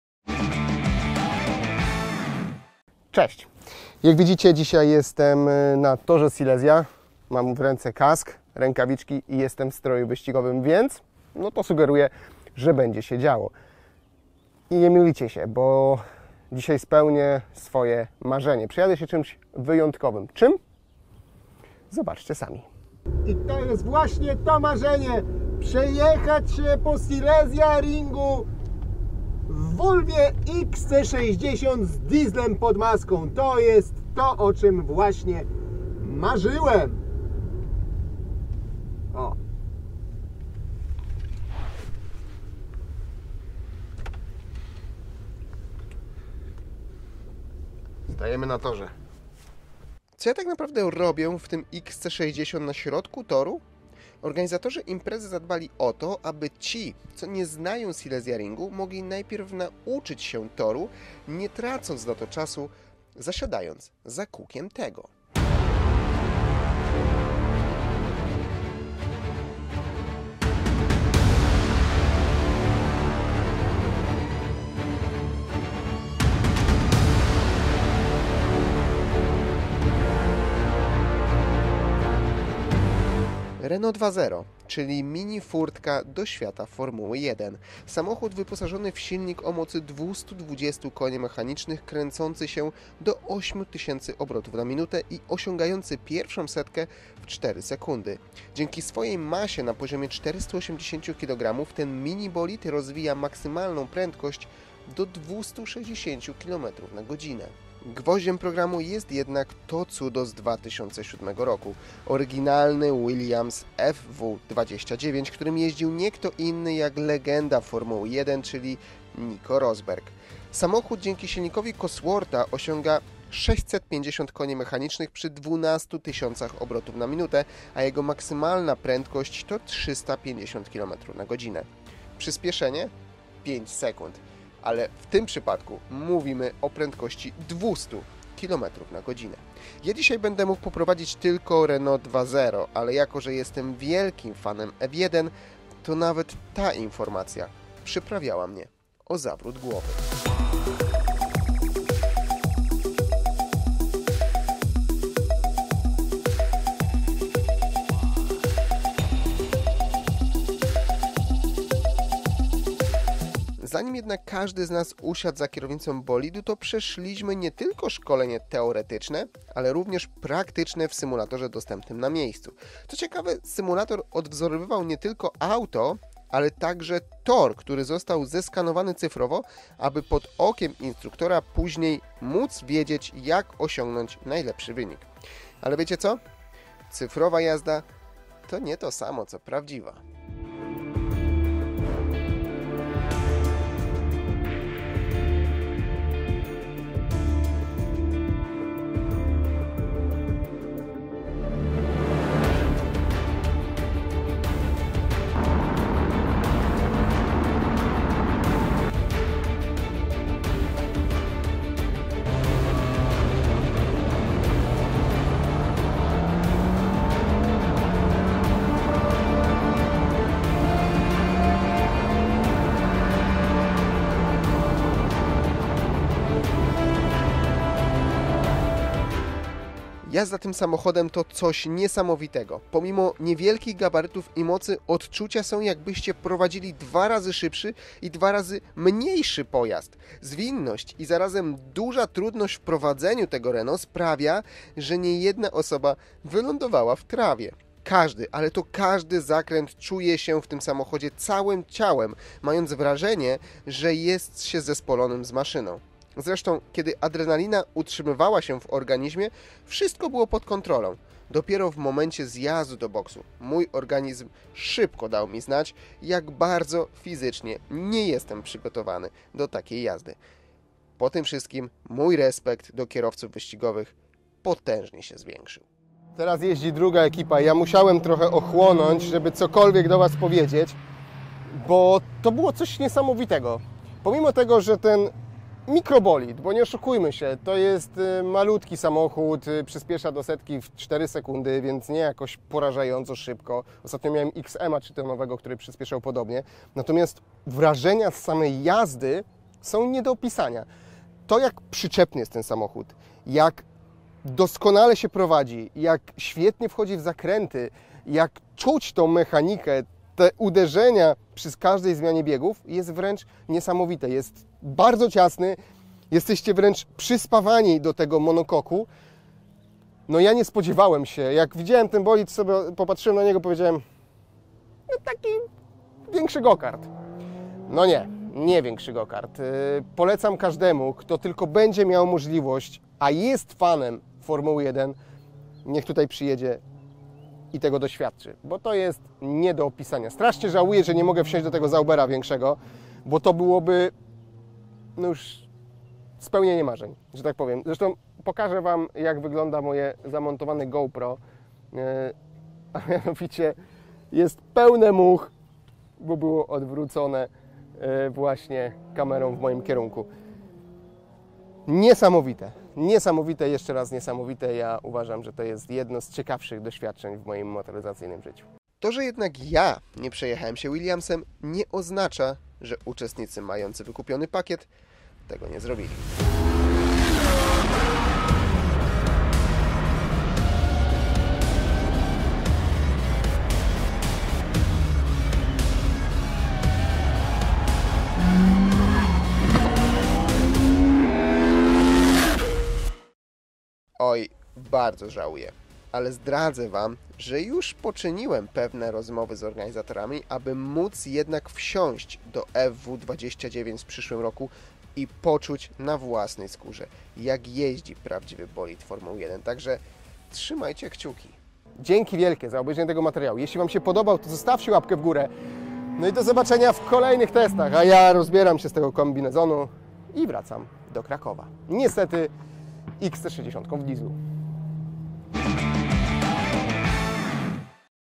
Formula Drive - doświadczenie inne niż wszystkie
Każdy fan Formuły 1 na myśl o samym zobaczeniu na żywo bolidu ma gęsią skórkę na całym ciele. Ja miałem możliwość nie tylko zobaczyć, ale także poprowadzić tą niesamowitą maszynę. Odczucia, jakie doświadczyłem za kierownicą, nie jestem w stanie porównać do niczego innego, a przez całą moją karierę motoryzacyjną miałem możliwość poprowadzić nie jeden wyczynowy samochód.  Jesteście ciekawi, jaka jest moja opinia? Zapraszam na film